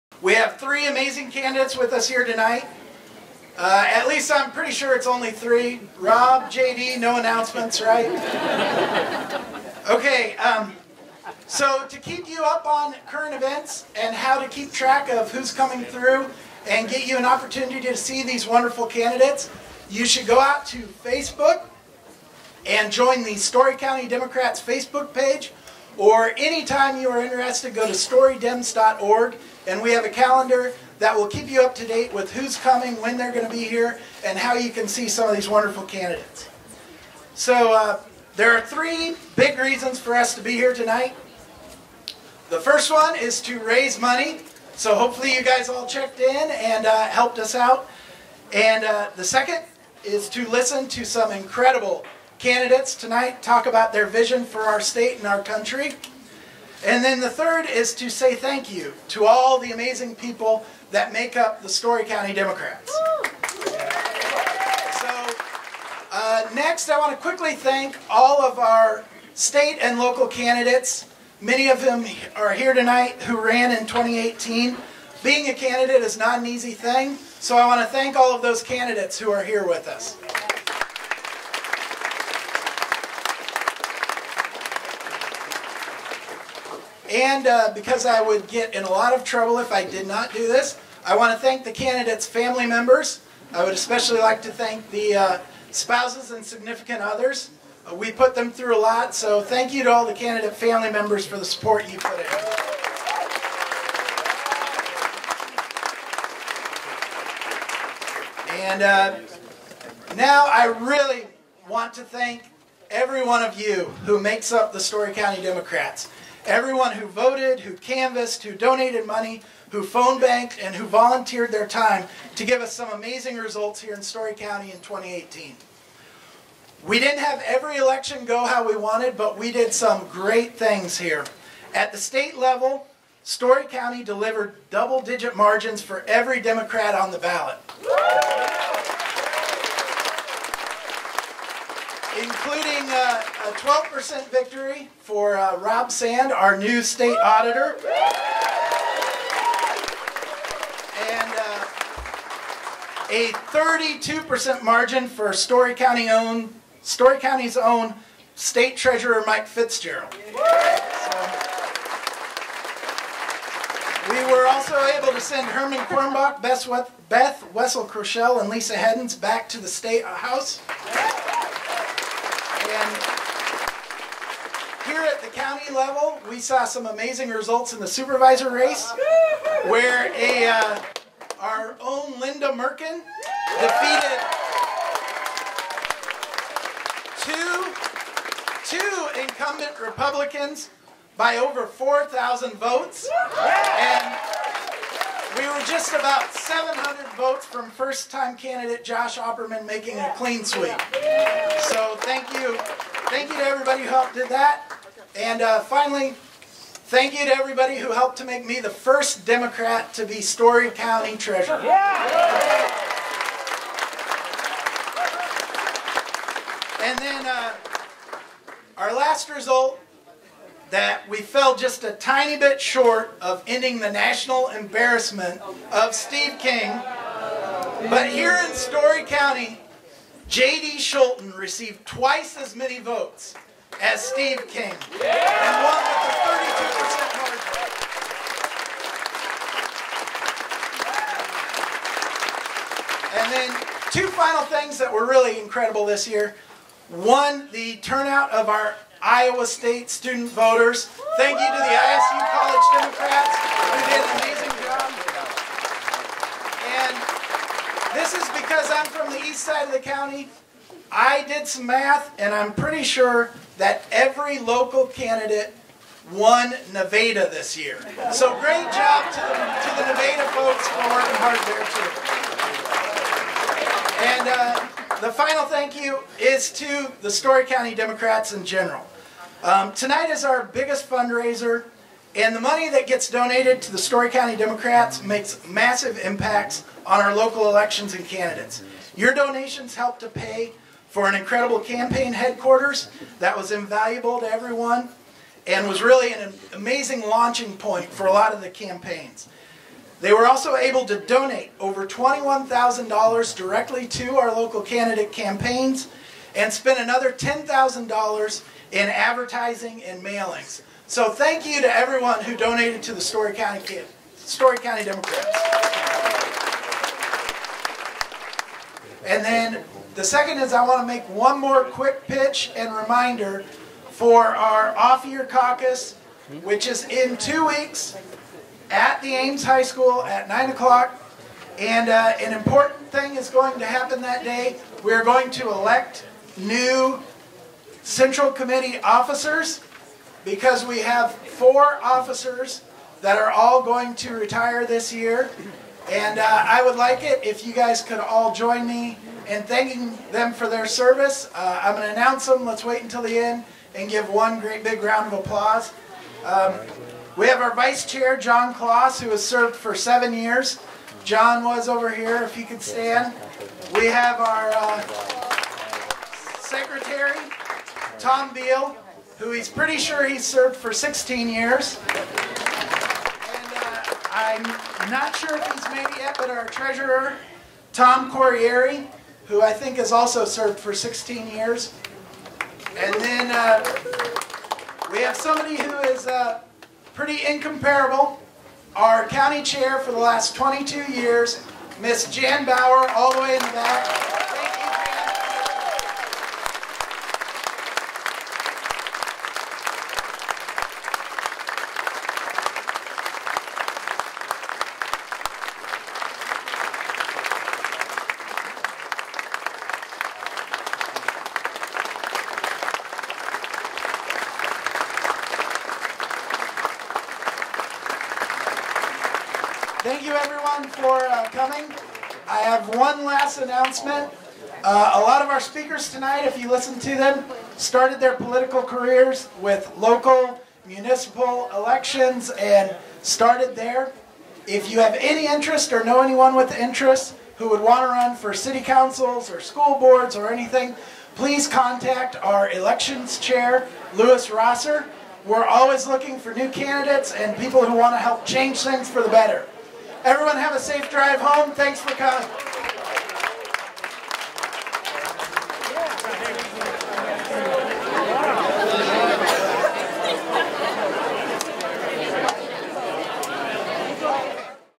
Ted Ramusson was the MC at the 2019 Story County Dems Soup Supper
Ted Rasmussen, Story County Treasurer, was the MC at the 2019 Story County Democrats Annual Soup Supper. He is newly elected and the first Democrat to attain the office of Story County Treasurer. This is a compilations of several moments where Ted spoke.